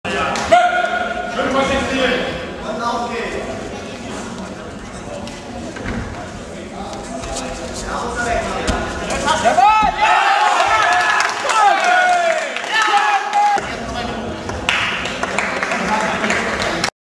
VfL Stade vs JKG Ostfriesland
2. Kampftag der  Judo Niedersachsenliga  2010 in Lüneburg